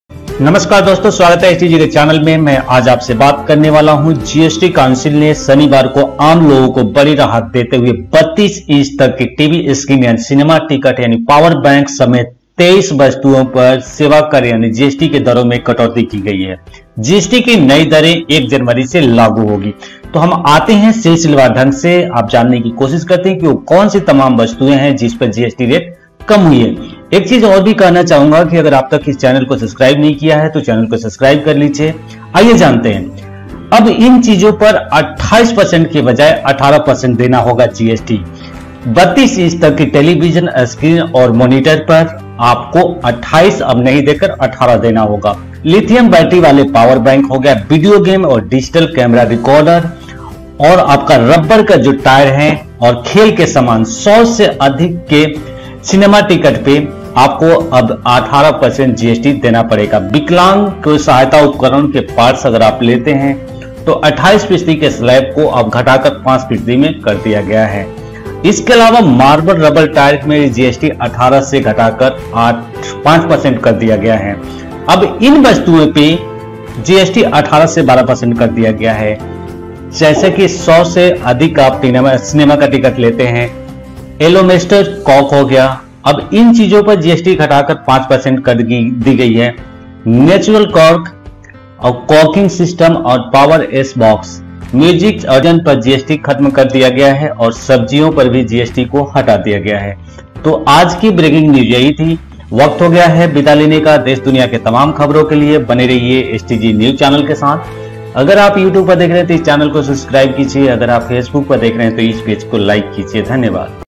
New GST Rate List: जानिए सरकार की राहत से कैसे पड़ेगा आपकी जेब पर असर
GST Council ने शनिवार को आम लोगों को बड़ी राहत देते हुए 32 इंच तक के टीवी स्क्रीन, सिनेमा के टिकट और पावर बैंक समेत 23 वस्तुओं पर वस्तु एवं सेवा कर (GST) की दरों में कटौती कर दी है. GST  की ये नई दरें एक जनवरी से लागू होंगी.

नई दिल्ली के विज्ञान भवन में Council की 31वीं बैठक के बाद वित्तमंत्री अरूण जेटली ने इन फैसलों की घोषणा की. उन्होंने कहा कि GST की दरें कम करने के इस फैसले से सालाना राजस्व में 5,500 करोड़ रुपये का असर पड़ेगा.

Council ने GST के 28 प्रतिशत के अधिकतम टैक्स के दायरे में आने वाली वस्तुओं में से 6 को 18 फीसदी वाले स्लैब में डाल दिया है. इसके साथ ही 28 प्रतिशत के स्लैब में अब केवल 28 वस्तुएं बची हैं.

वित्त मंत्री ने बताया कि सिनेमा के 100 रुपये तक के टिकटों पर अब 18 प्रतिशत की बजाय 12 प्रतिशत की दर से और 100 रुपये से ऊपर के टिकट पर 28 प्रतिशत की बजाय 18 फीसदी की GST लगेगा. पढ़िए आखिर किन-किन चीजों पर कितना-कितना जीएसटी घटाया गया.

अब इन चीजों पर 28% की बजाय 18% देना होगा GST

- 32 इंच तक की टेलीविजन स्क्रीन्स और मॉनिटर

- लीथियम बैटरी वाले पावर बैंक

- वीडियो गेम और डिजिटल कैमरा रिकॉर्डर

- रि-ट्रीटेड रबर टायर

- खेल के सामान

- 100 से ज्यादा की कीमत वाले सिनेमा टिकट

विकलांगों के सहायक उपकरण के पार्ट्स पर लगने वाले GST के 28 फीसदी के स्लैब को घटाकर 5 फीसदी किया गया. इसके अलावा मार्बल रबर टायर पर जीएसटी की दर 18 से घटाकर 5 फीसदी कर दी गई है.

इन वस्तुओं पर GST 18 से 12% किया गया

- एग्लोमेरेटेड कॉर्क

- 100 तक के सिनेमा टिकट

इन चीजों पर GST दर घटाकर 5% की गई

- नेचुरल कॉर्क

- वॉकिंग स्टिक

- फ्लाई ऐश ब्लॉक्स

म्यूजिक बुक और बेजिटेबल पर जीएसटी खत्म कर दिया गया है. अभी तक म्यूजिक बुक पर 12 फीसदी और बेजिटेबल पर 5 फीसदी का जीएसटी लगता था.